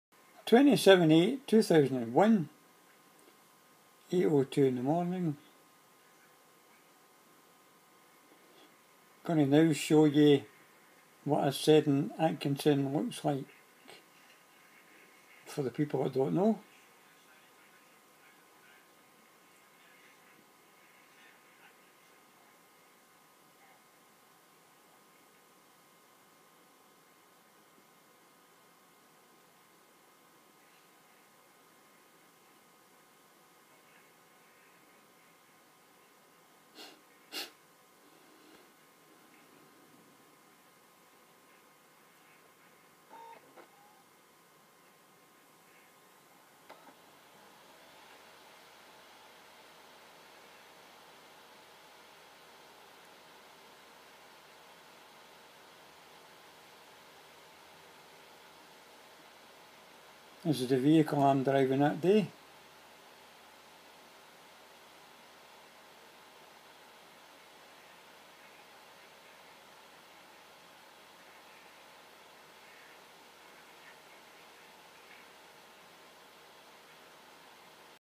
Seddan Atkinson At Camelon Aug 2001
View of seddan Atkinson unit for non truckers this one has 4 gears split into 3 sections making 3 changes on each gear G405 RUS